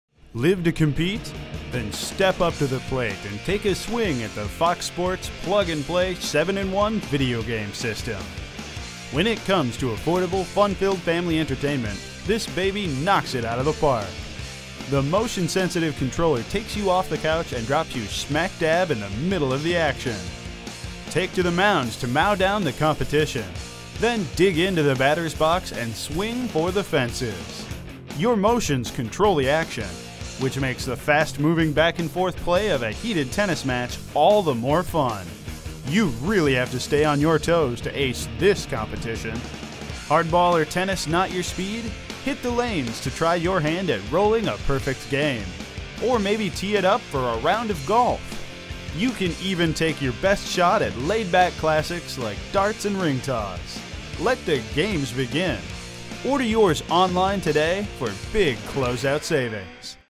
FOX Sports Video Game
Serve up a fastball to your buddy in baseball, or maybe a round of golf against the computer is more your style. Either way, you'll get 7 classic games like bowling, darts, and tennis with sharp 16-bit graphics. It's all controlled by the motion-sensitive controllers, which means that when you play golf, you actually swing the controller!

It's a great way for kids and adults to stay active, and it requires no additional hardware... just plug the System into the your television's AV input and you're ready to test your mettle against the computer or a friend. Best part? Manufacturer's overstock brings this sweet System to you at a fantastic price!